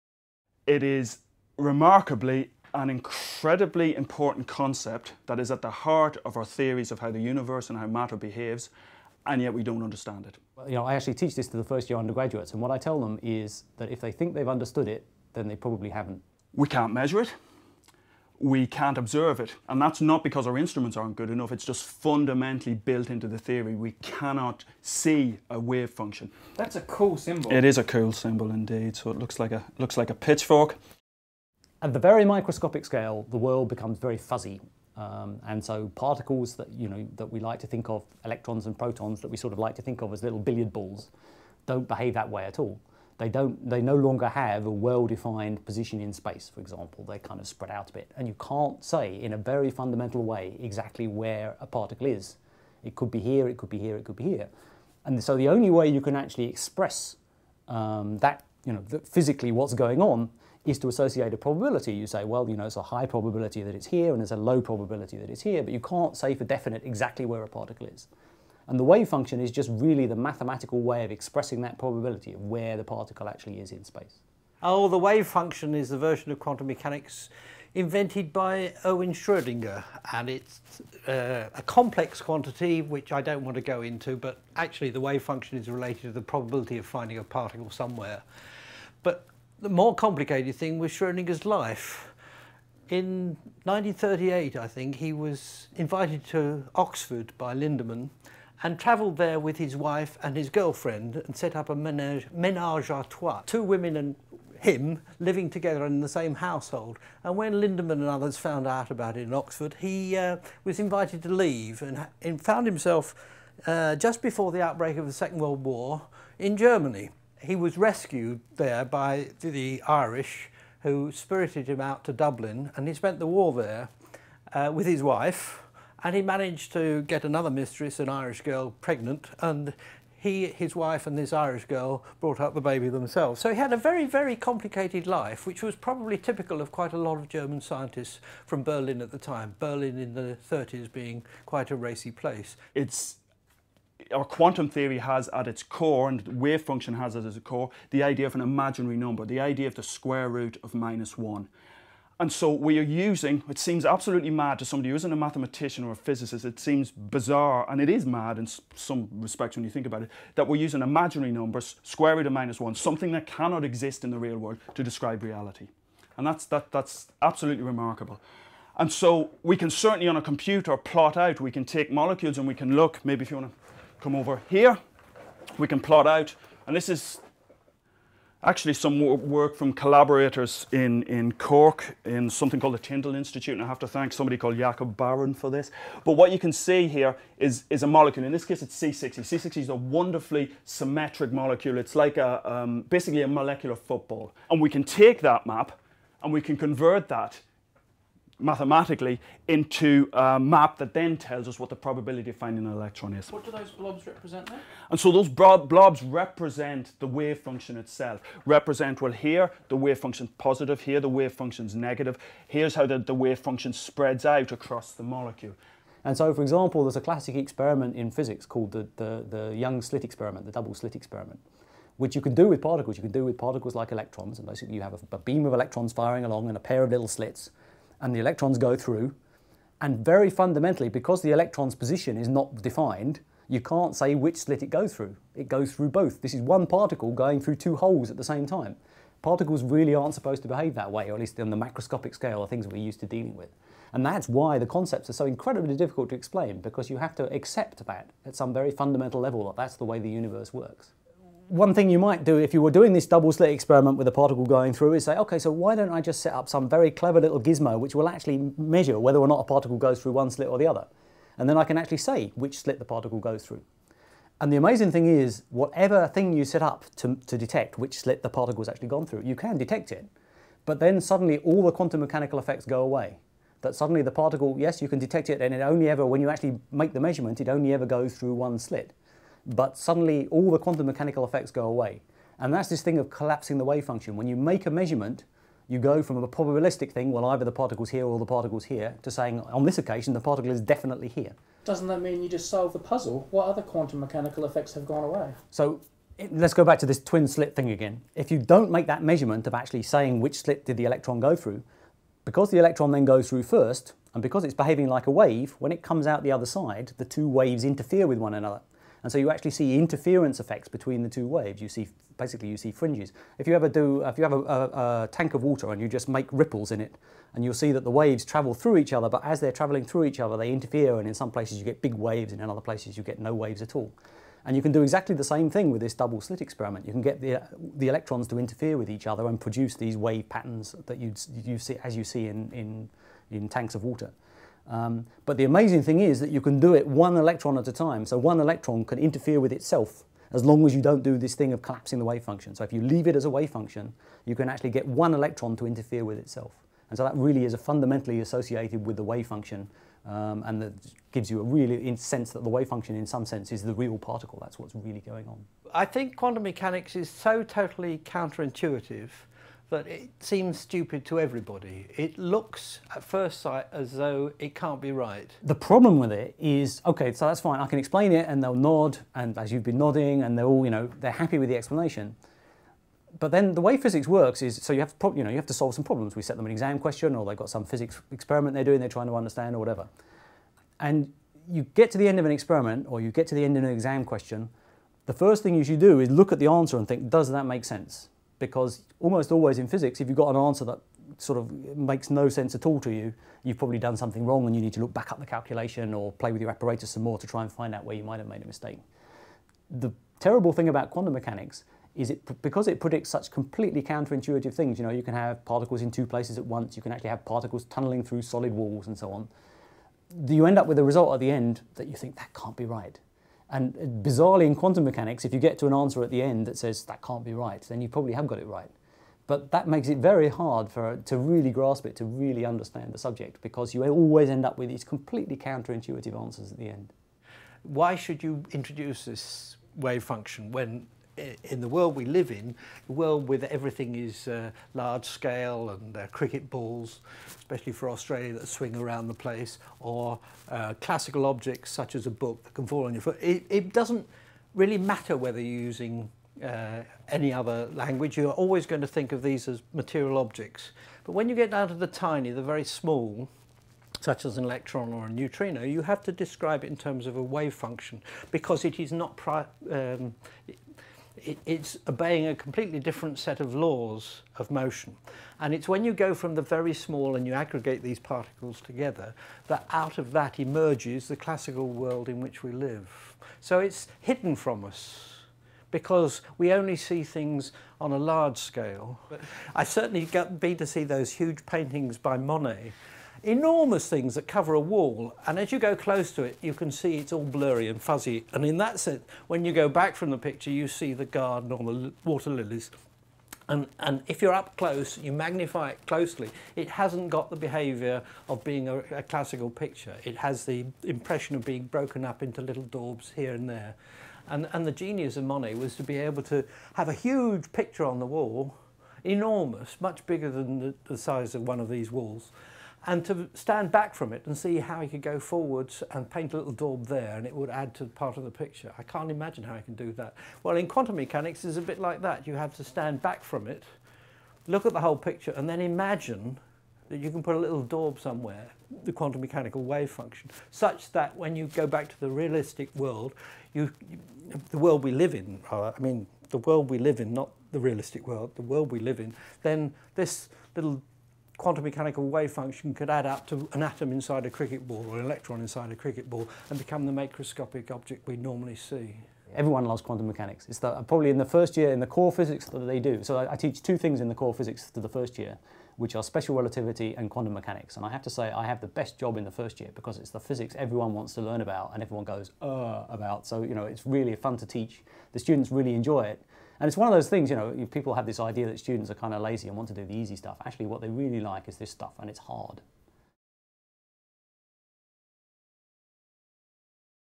Wave Function - Sixty Symbols
If you think you understand this video, you probably don't. Another adventure into the world of quantum mechanics with three professors of physics and astronomy.

With Professor Mike Merrifield